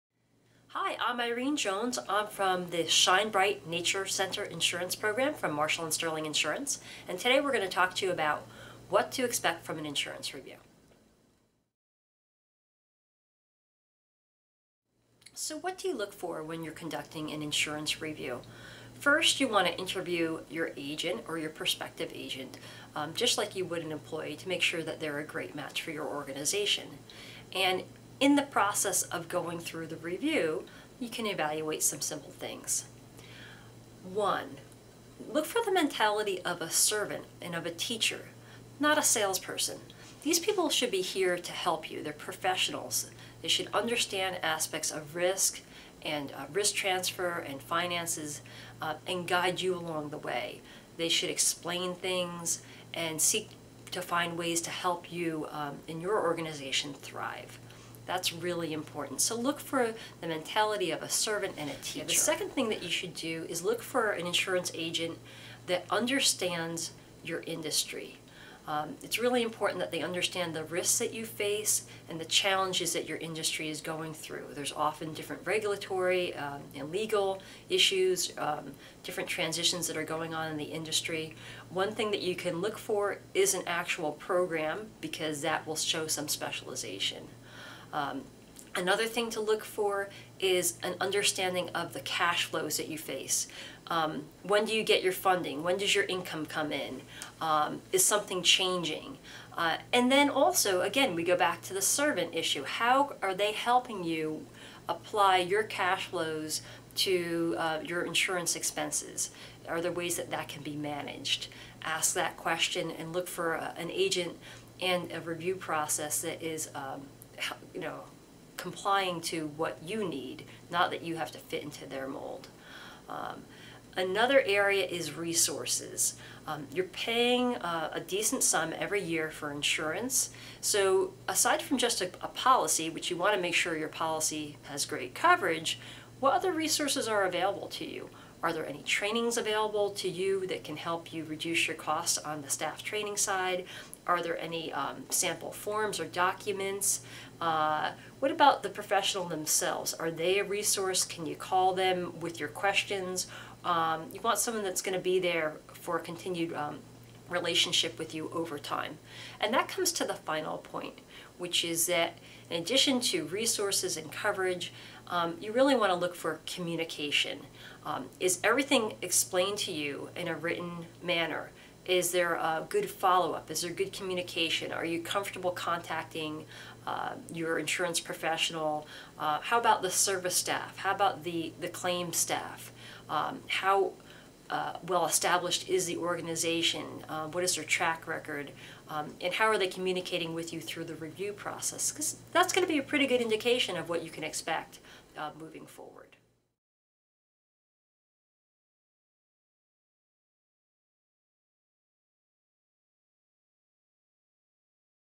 What to Expect When Conducting an Insurance Review- Shine Bright Nature Center Insurance Program
Irene Jones, Program Development Manager of Marshall & Sterling's Shine Bright Insurance Program, shares what to expect when conducting an Insurance Review.